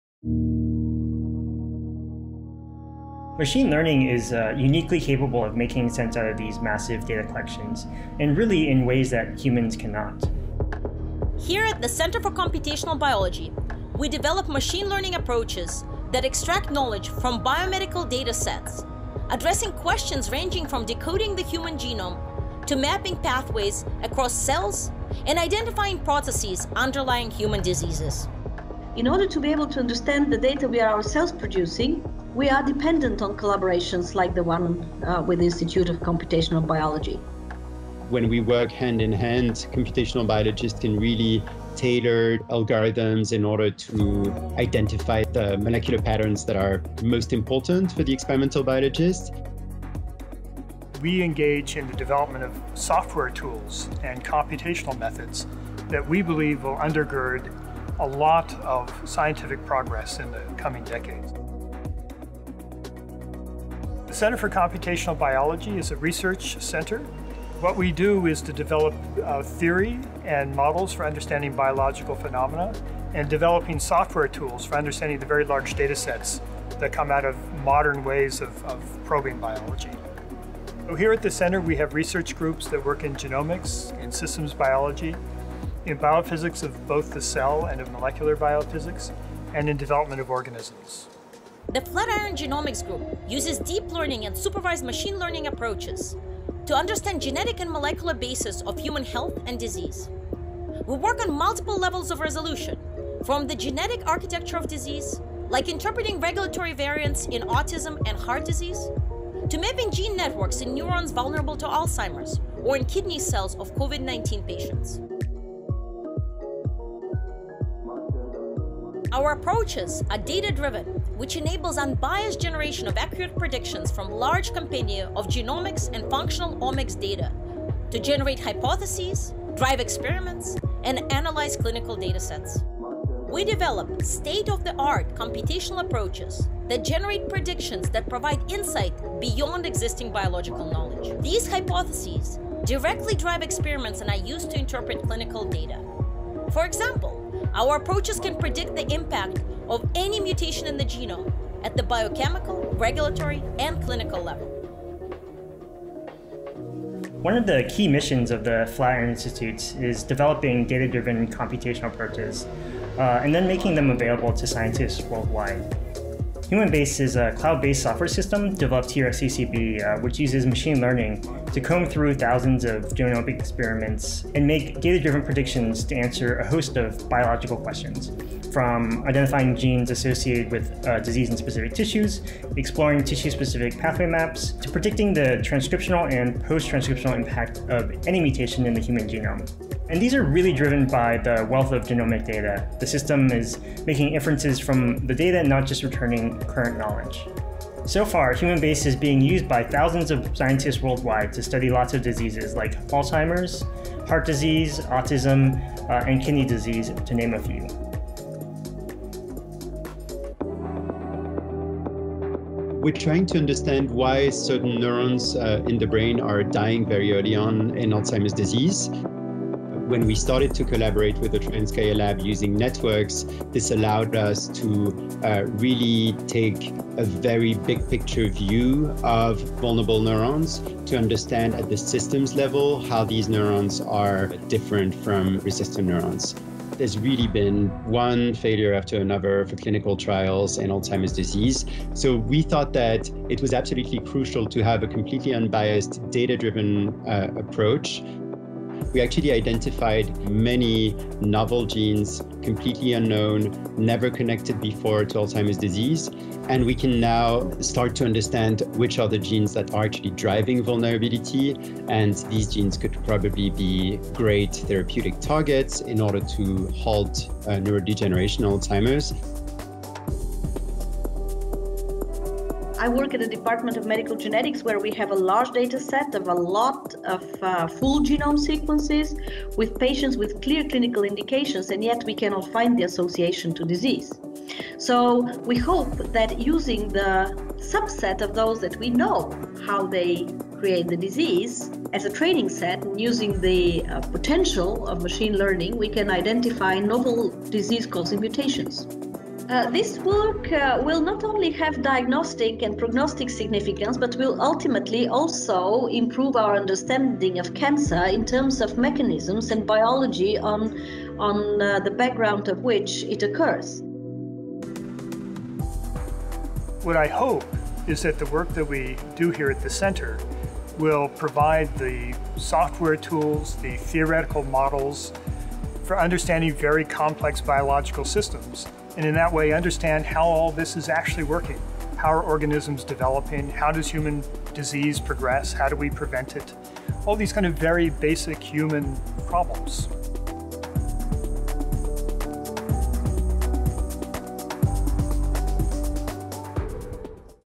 The Simons Foundation, Inc. (through its Center for Computational Biology at the Flatiron Institute)
Data-driven Discovery in Human Genomics and Medicine.

Recent developments in medicine and biology have given rise to massive amounts of biological data. The Flatiron Institute's Center for Computational Biology develops advanced computational methods and software systems for making sense of these vast data collections.  Through the development of data-driven computational approaches and close collaboration with experimentalists and clinicians, the CCB Genomics group addresses biomedical problems at multiple levels of resolution - from deciphering the genetic architecture of disease, including interpreting regulatory variants in autism and heart disease, to mapping functional networks in neurons vulnerable to Alzheimers or in kidney cells of COVID-19 patients. A key mission of CCB is to make these state-of-the-art computational methods available to biomedical scientists worldwide - this is exemplified in Humanbase.io, a cloud-based, interactive system for data-driven predictions in human molecular biology.